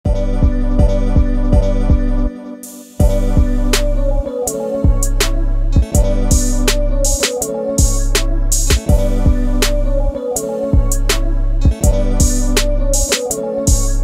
Sandbar sharks disappearing into a giant bait ball as they attempt to feed.🦈
Hi i post random tiktok videos as